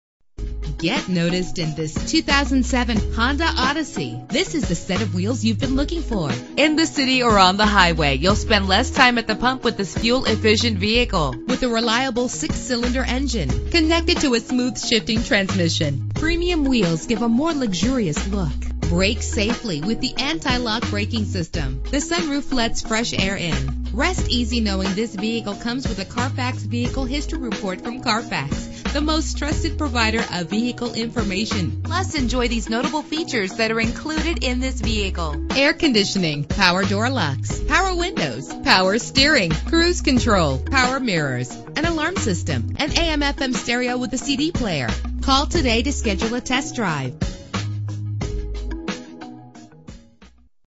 2007 Honda Odyssey - Huntington NY
This vehicle represents similar vehicles available at Huntington Honda.
for availability of specific vehicles.
Phone:
Year: 2007
Make: Honda
Model: Odyssey
Trim: EX-L
Engine: 3.5 liter 6 cylinder
Transmission: AUTO 5SPD
Color: Slate Green Metallic
Mileage: 35128
Address:
1055 E. Jericho Tpke